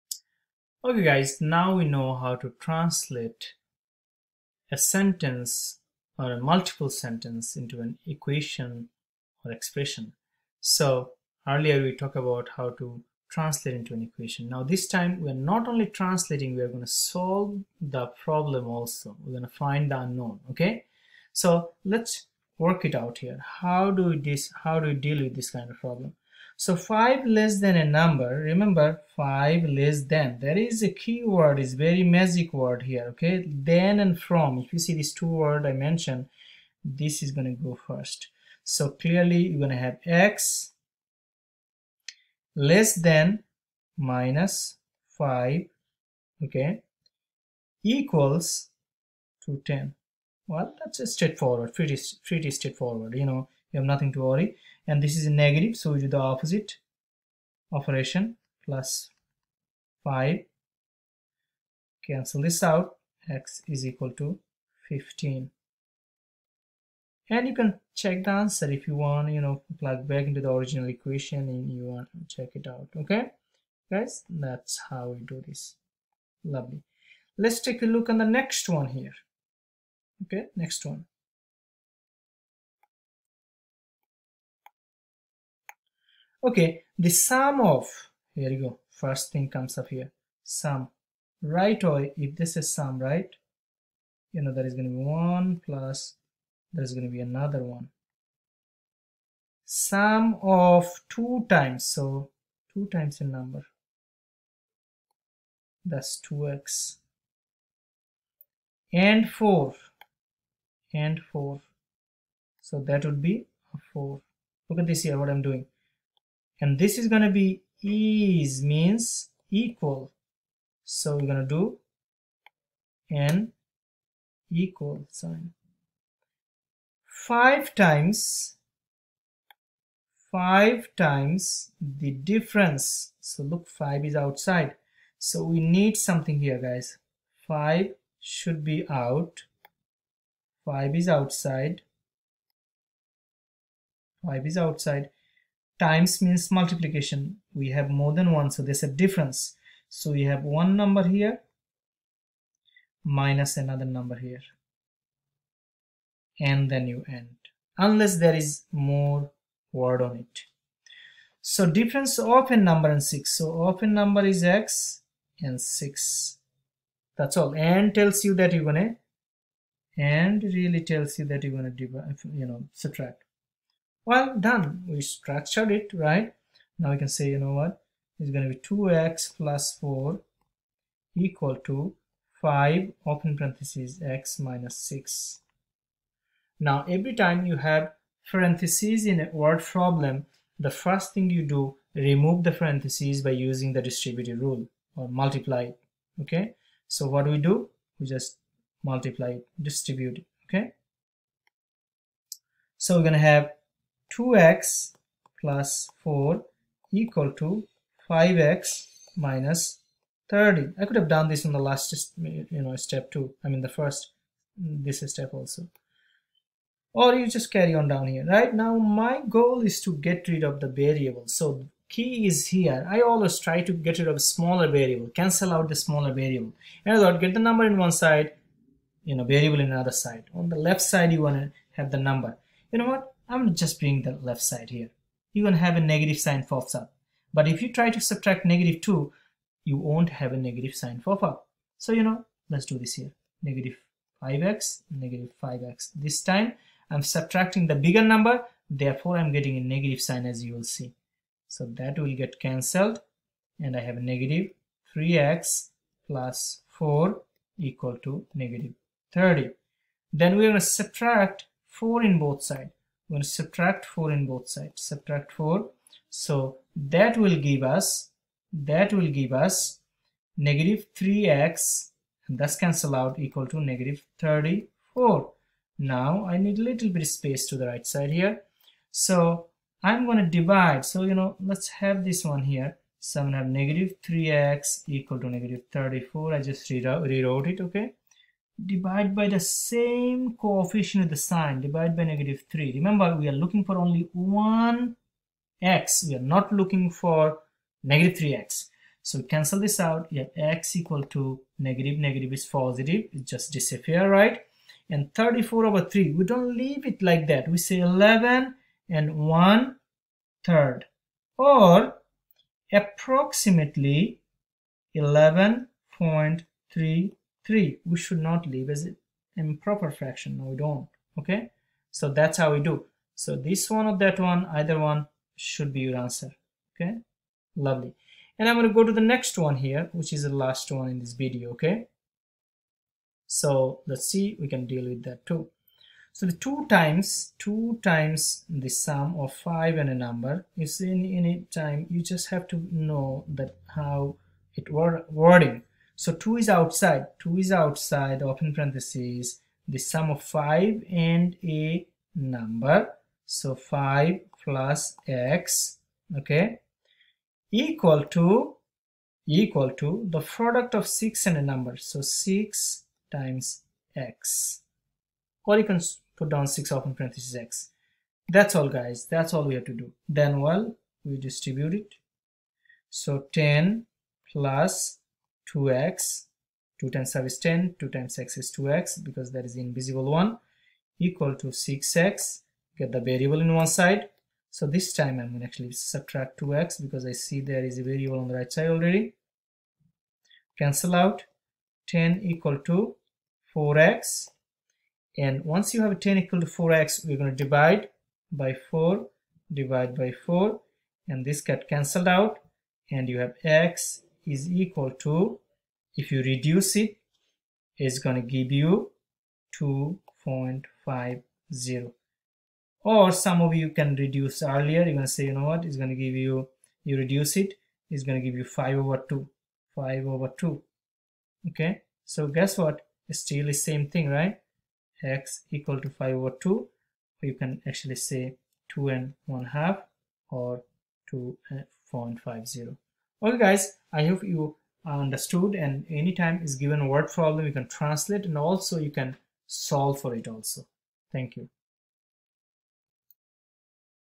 Translating and Solving problem in Algebra Part 1
This video will teach you how to solve problems with algebra.